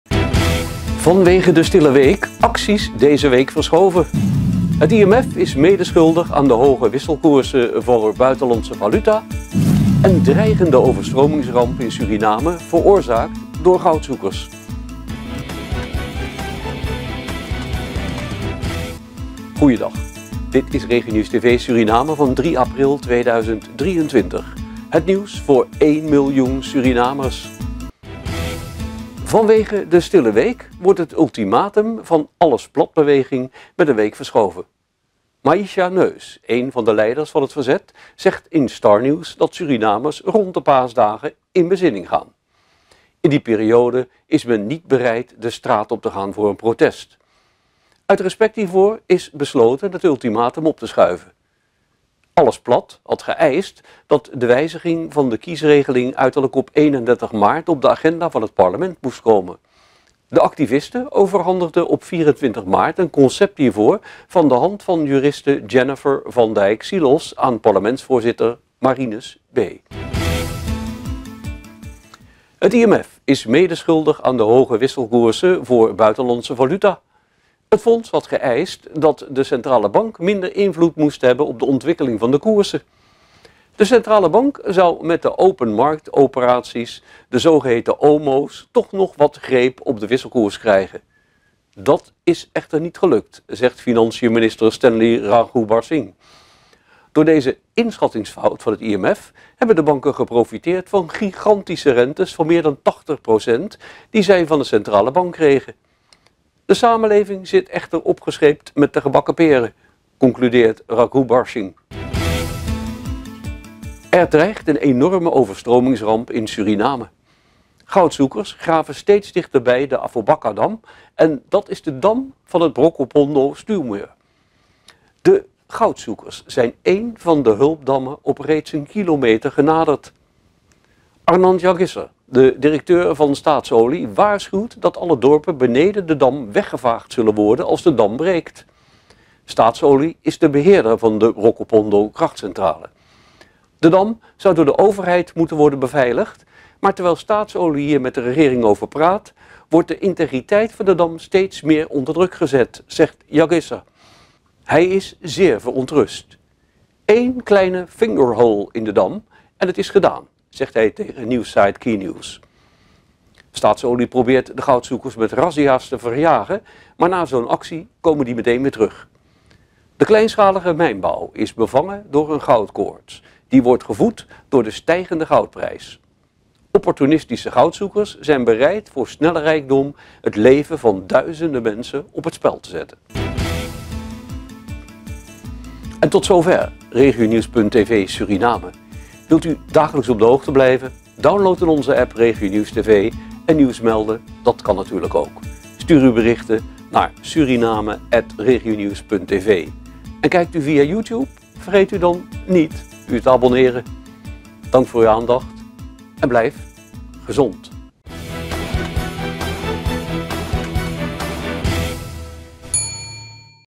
Regionieuws TV Suriname -IMF medeschuldig hoge wisselkoersen -Goudzoekers veroorzaken overstroming?
- Vanwege de 'Stille Week' acties deze week verschoven
- IMF is medeschuldig aan de hoge wisselkoersen voor buitenlandse valuta
- dreigende overstromingsramp in Suriname veroorzaakt door Goudzoekers